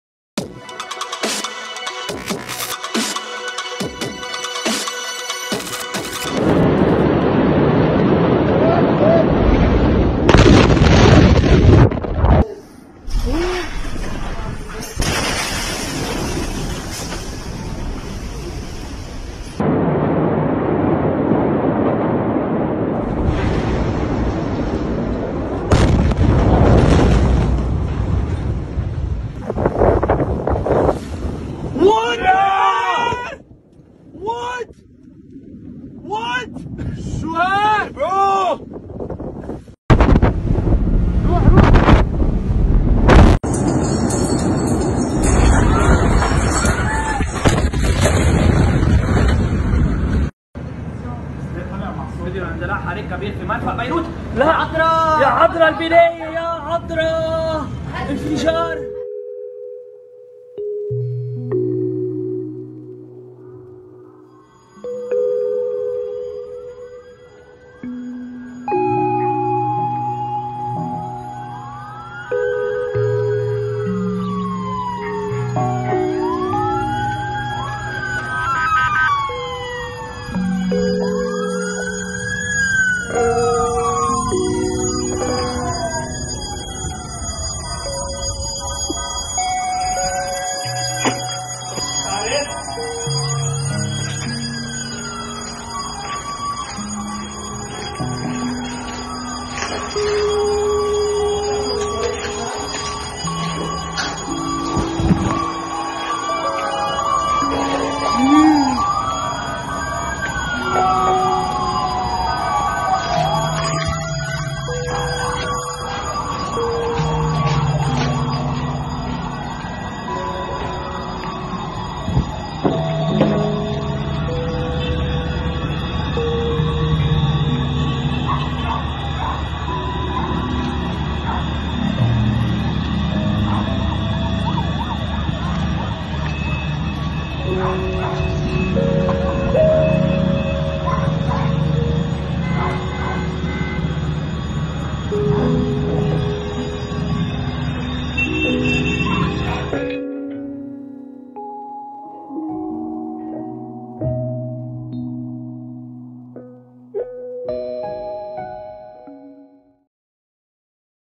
7 Angles of Beirut Blast!!
August 4, 2020 - A powerful explosion rocked Beirut, the Lebanese capital just now.Reports of casualties are still coming in according to Lebanon's health ministry.The explosions come at a time when Lebanon is suffering its worst economic crisis in decades, which has left nearly half of the population in poverty.  Still, no one is sure what caused the massive blast that shook Beirut. Here is the explosion from seven different angles.

Pray4Beirut